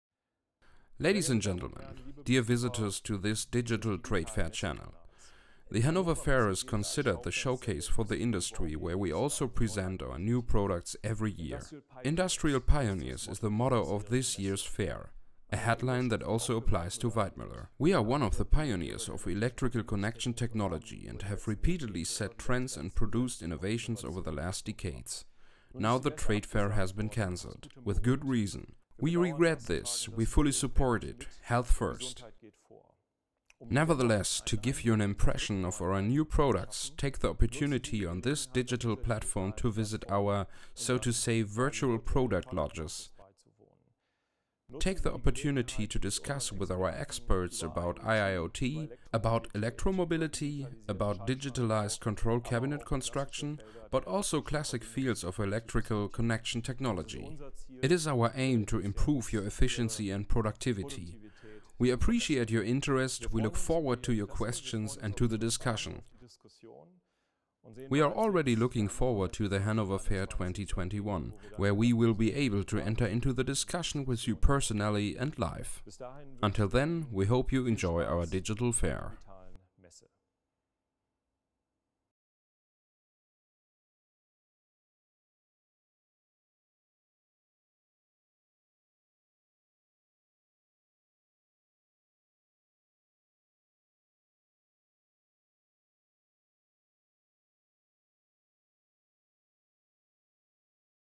Digital trade fair experience 2020 - Pioneering Industrial (ENG)
Our CEO and Chief Technology Officer Volker Bibelhausen welcomes you to our digital offer instead of the Hannover Messe and gives you an overview of our highlight topics.